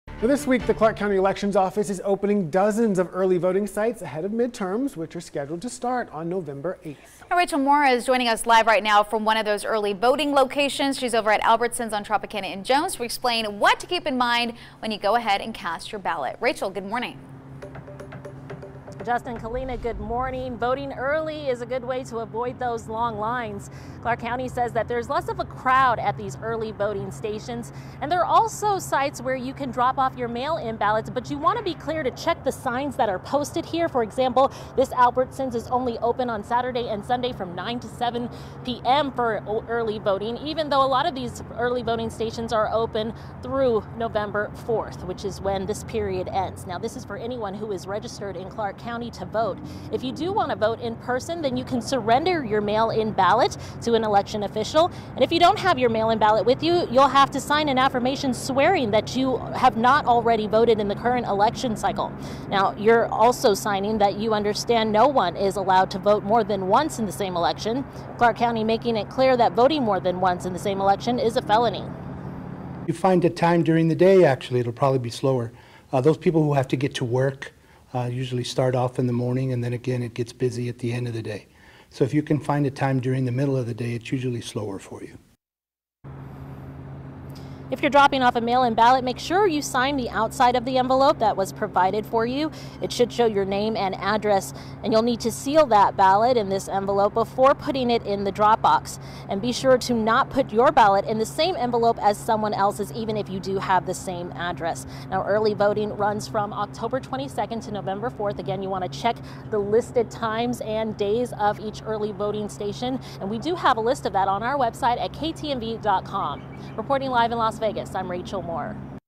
Early Voting in Clark County open October 22
Early Voting in Clark County runs from October 22 to November 4. Clark County election officials encourage voters to visit an early voting site to avoid long lines and crowds.